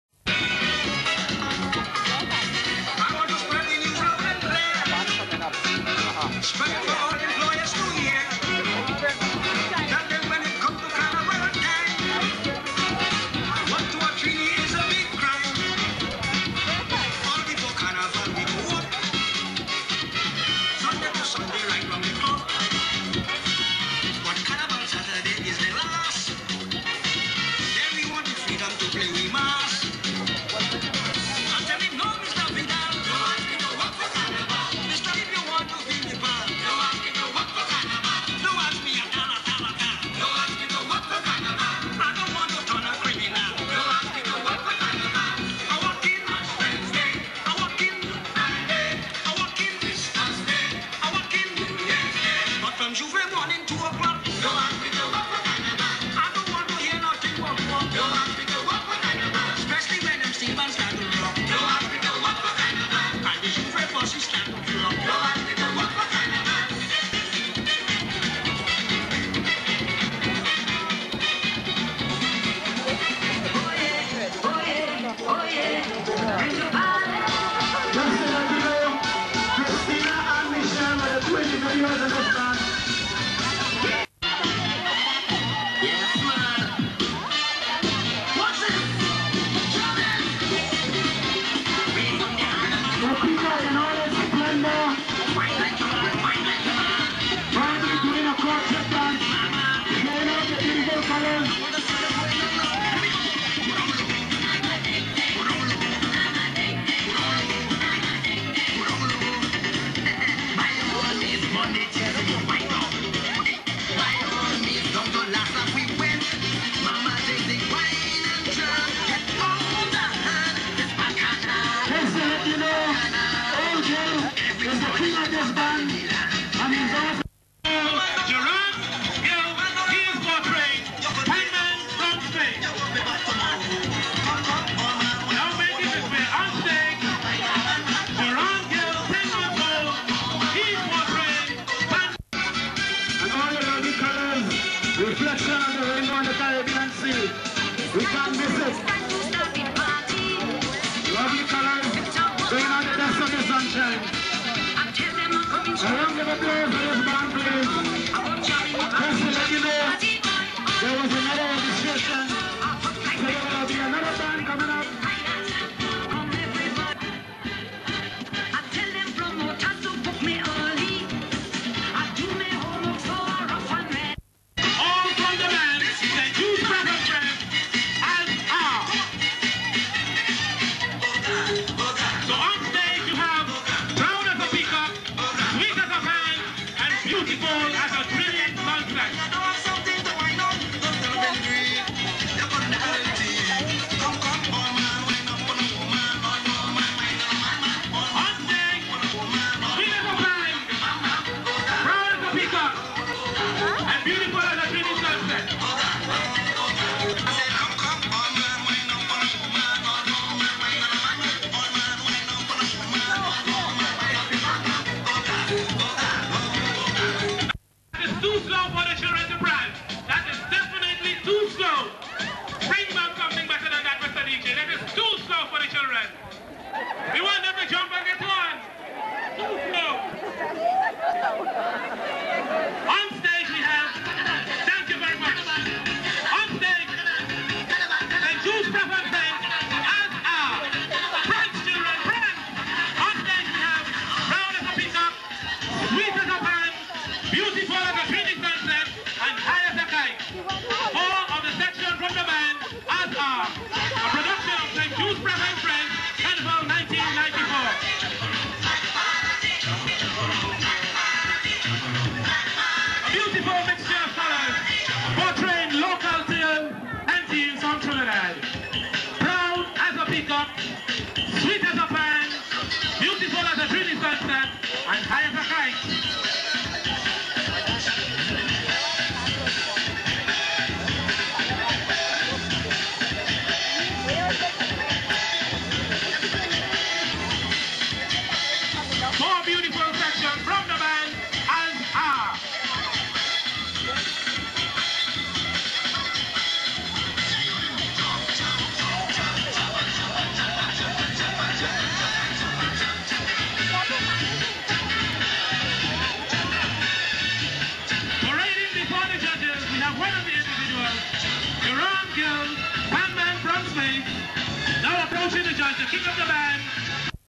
Kids Carnival - Trinidad 94
Local Kids Carnival. Location - Sevilla, Trinidad.
Date Video shot - 1994.